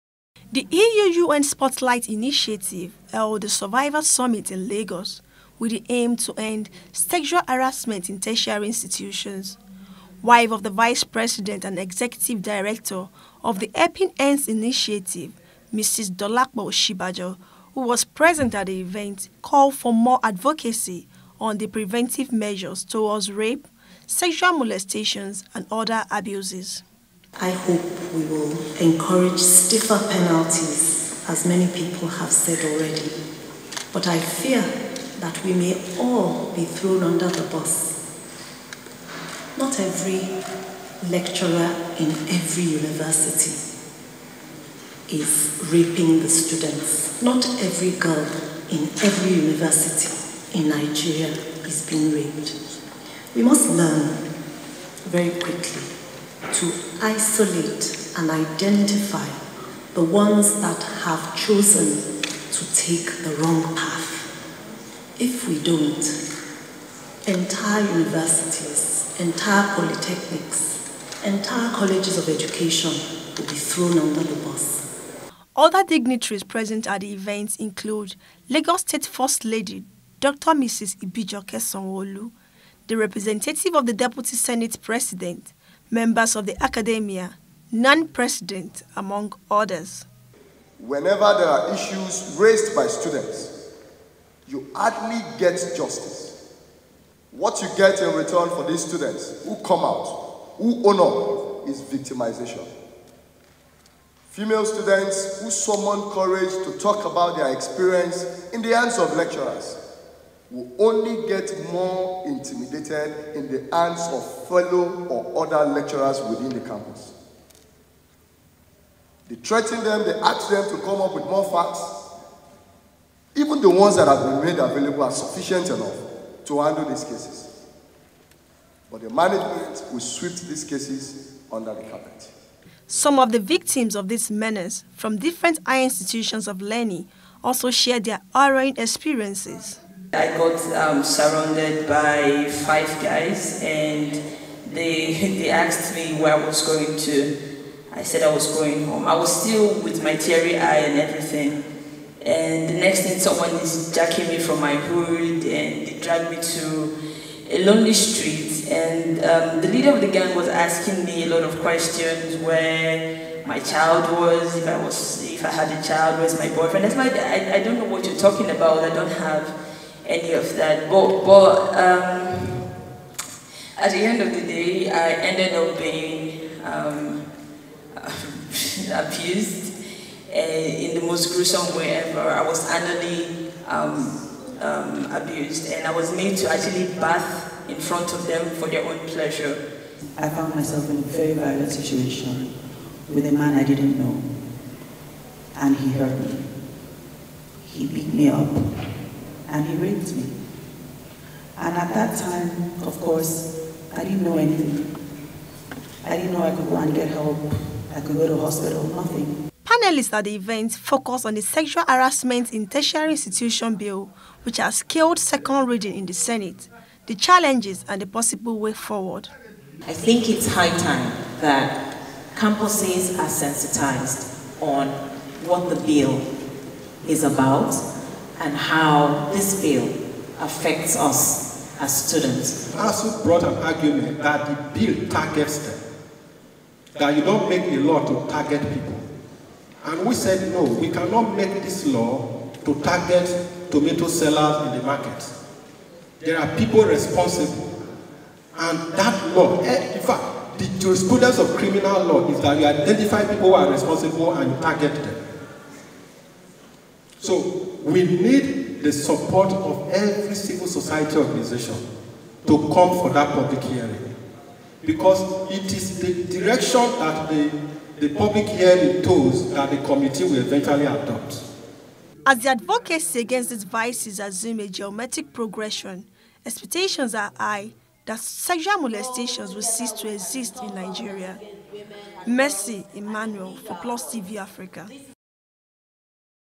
Survivor Summit - Ending Sexual Harassment In Tertiary Institutions
In a quest to support a Nigeria where all women and girls, particularly the most vulnerable, live a life free from violence and harmful practices, the UN in Nigeria under the Spotlight Initiative to end violence against women and girls with focus on the tertiary institutions held a Survivors’ Summit in Lagos.

OUR CORRESPONDENT, MERCY EMMANUEL WAS THERE.